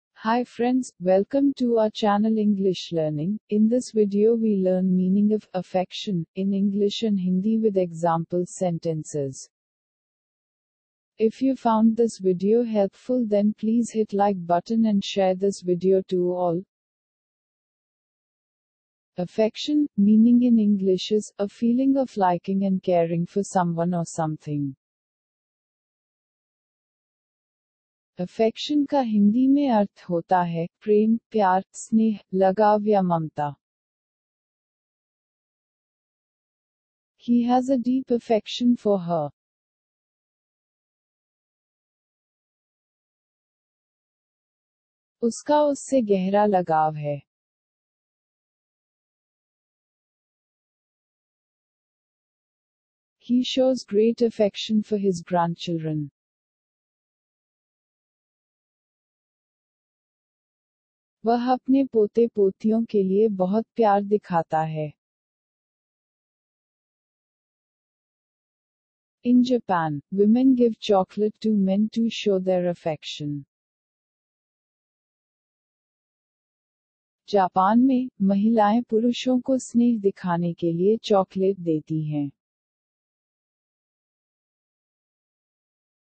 Meaning of Affection in Hindi and English
Hi Friends,
In this video we learn meaning of affection in English and Hindi with examples sentences.
affection, meaning in English is, a feeling of liking and caring for someone or something
affection का हिंदी में अर्थ होता है, प्रेम, प्यार, स्नेह, लगाव या ममता
He has a deep affection for her.
उसका उससे गहरा लगाव है।
He shows great affection for his grandchildren.
वह अपने पोते-पोतियों के लिए बहुत प्यार दिखाता है।
In Japan, women give chocolate to men to show their affection.
जापान में, महिलाएँ पुरुषों को स्नेह दिखाने के लिए चॉकलेट देती हैं।